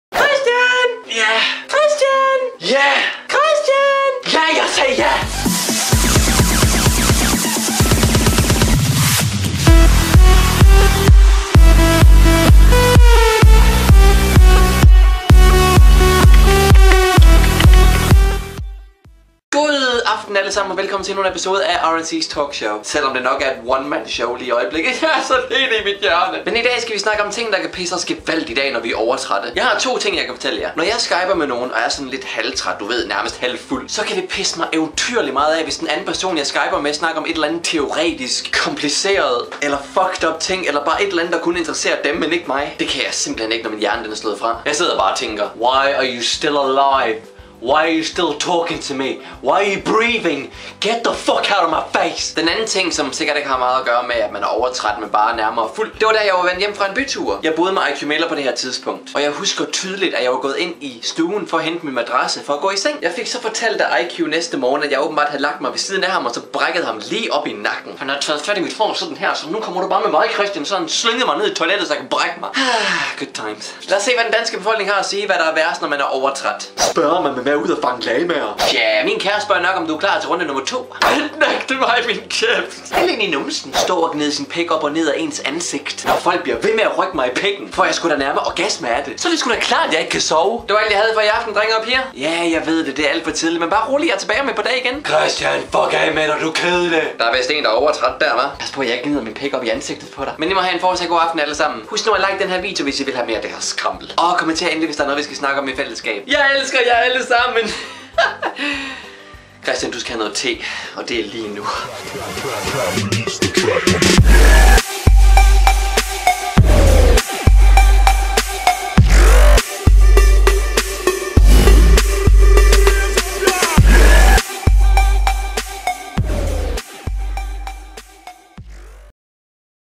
Overtræt
BESKRIVELSE: I denne serie snakkes der om random emner! I dag er det om, at være for overtræt!

1154 København K
Attn.: R&C